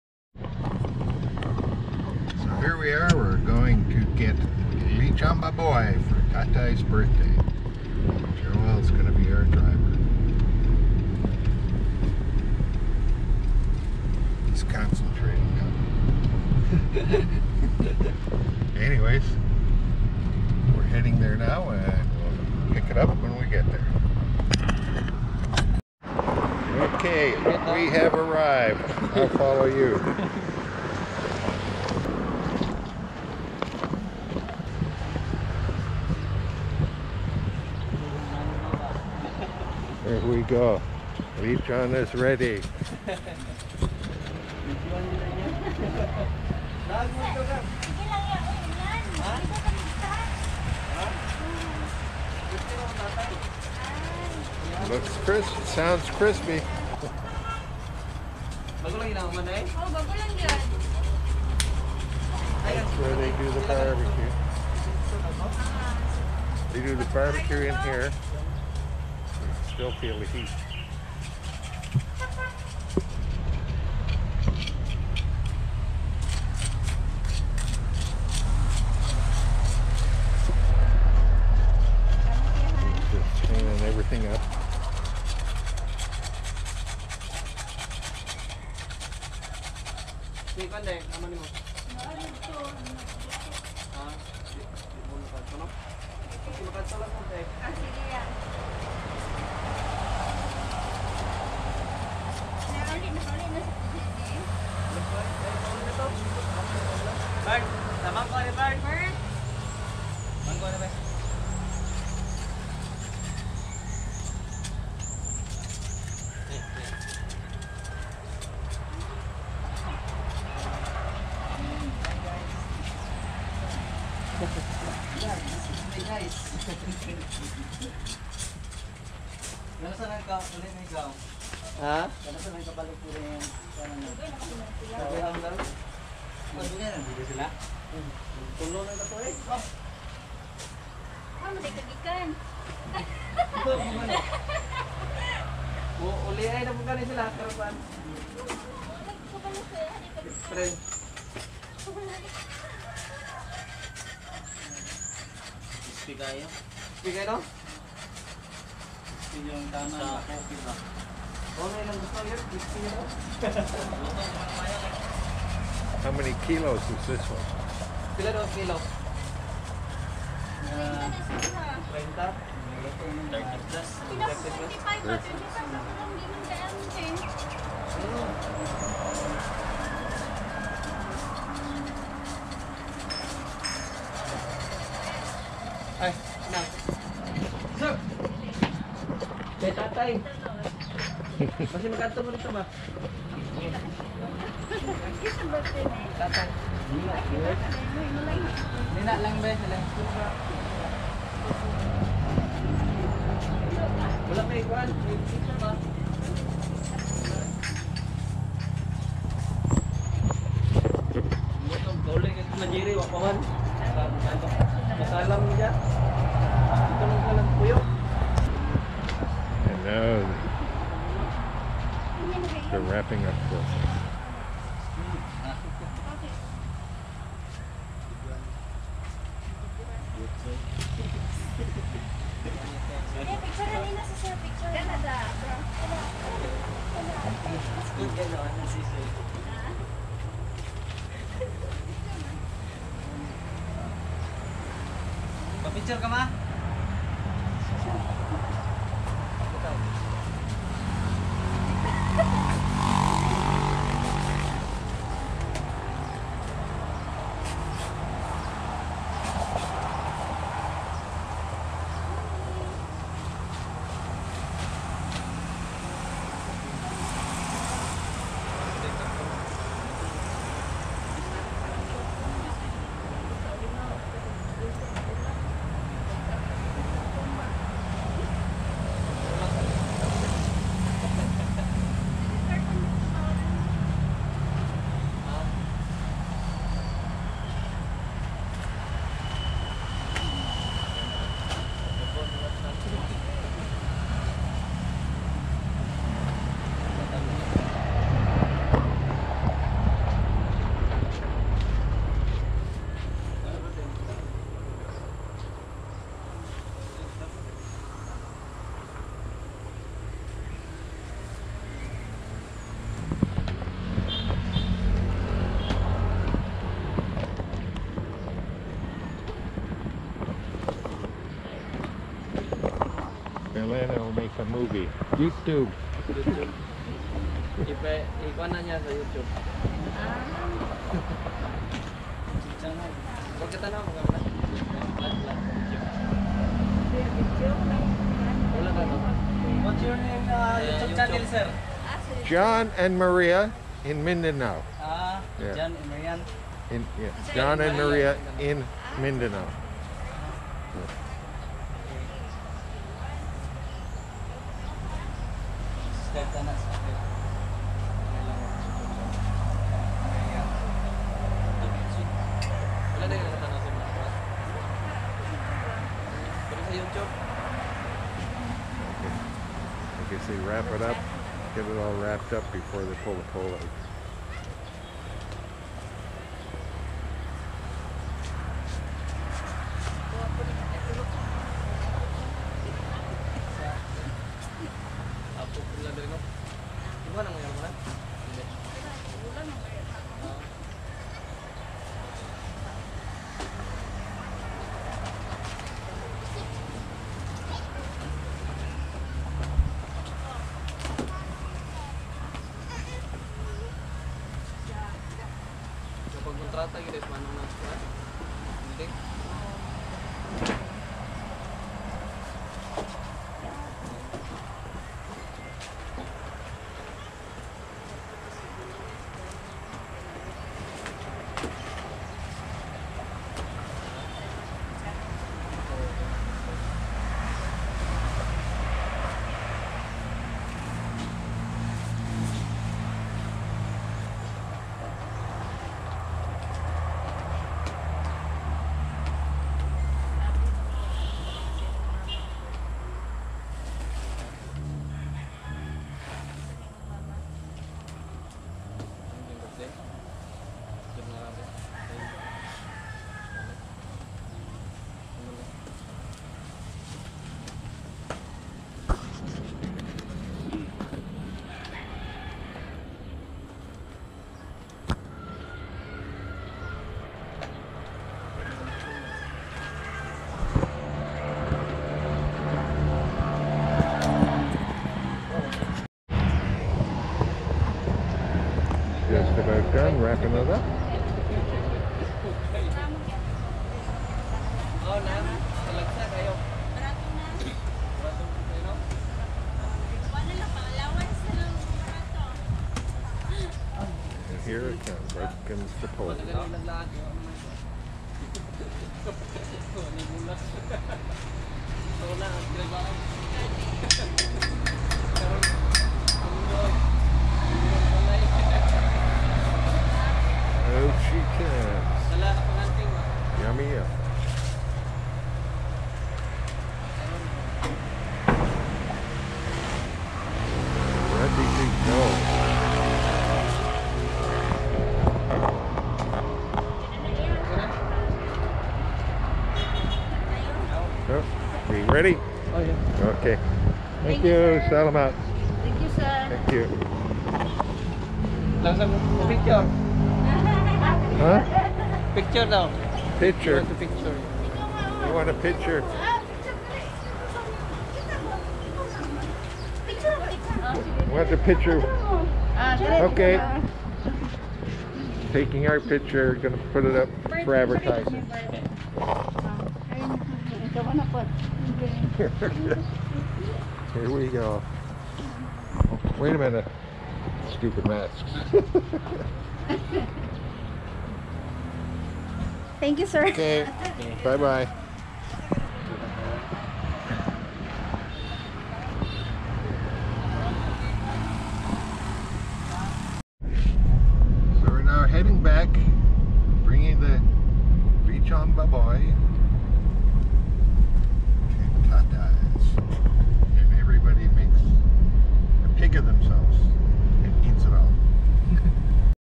HOW A LECHON IS PREPARED AND WRAPPED FOR DELIVERY HERE IN THE PHILIPPINES. YUMMY LECHON, KIDAPAWAN
When you order a lechon, you never know if it will be crispy or not and lechon is all about the skin. If it is crispy then it is worth the money. If it is not, then you might as well not have one. This one is good. Thanks to R and D Lechon House.